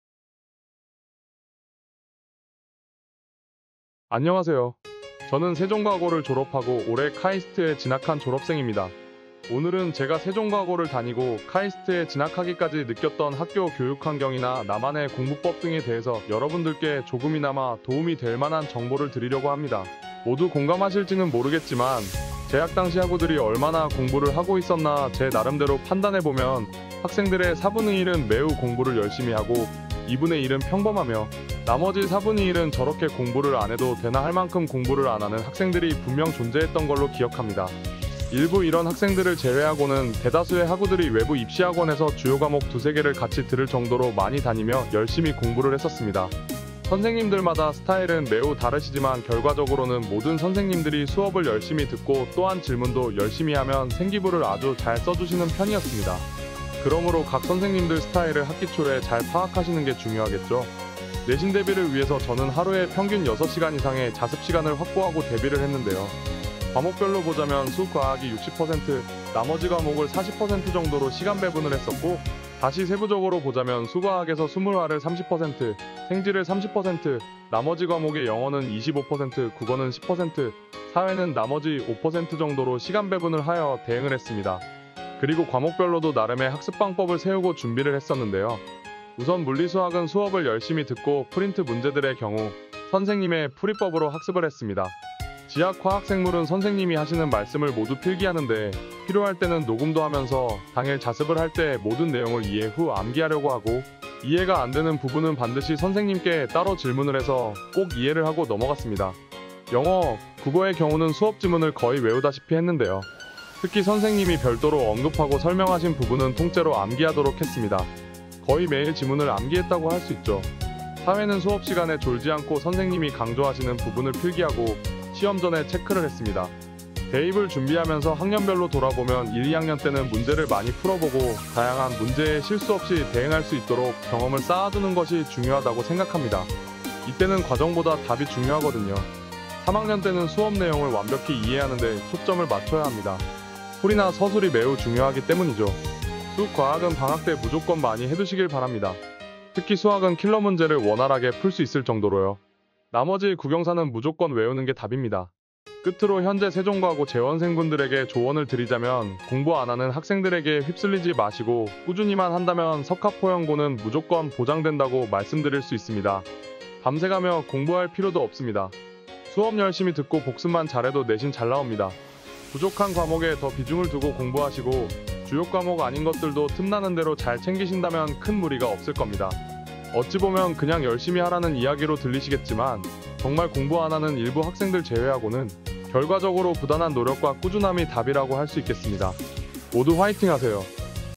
졸업생이 전해주는 세종과학고등학교
아우라에듀 출신이자 세종과고를 졸업해서 올해 카이스트에 진학한 학생이 전해주는 세종과고 이야기, 세종과고 졸업생은 어떻게 공부하고 학교생활은 어떻게 했을까?